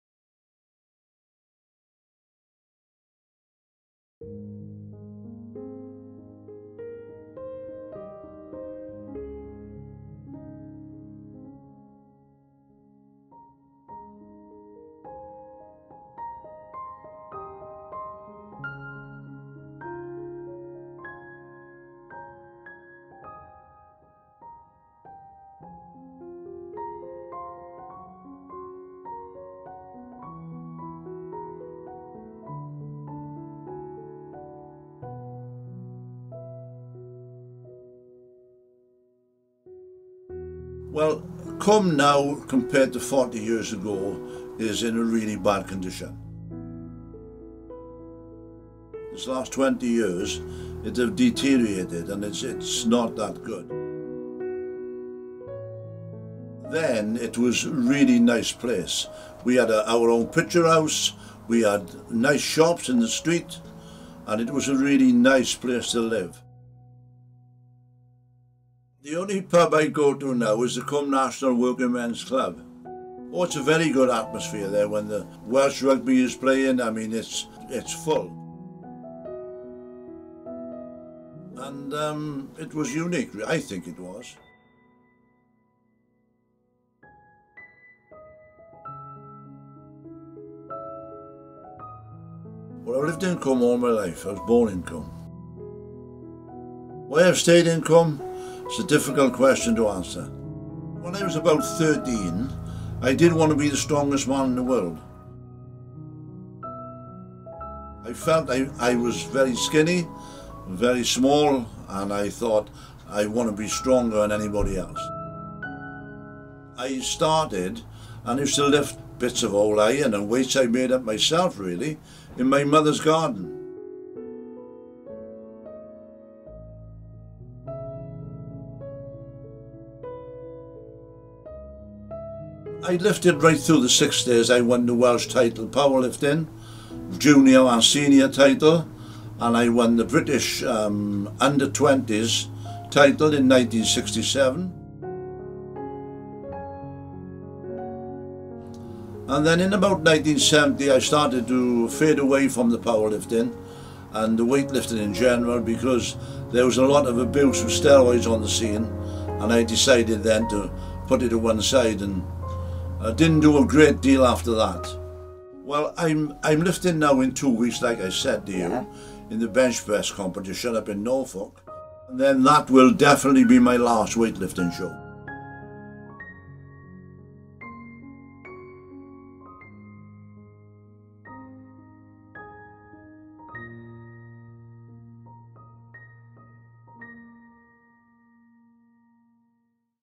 A Portrait of Cwm
This project is based on a small village in the heart of the  Welsh Valleys, Cwm, we interview a local man who has lived there all his life and we explore his life within the historical village.
Thanks to Jeremy Richards for the archive footage.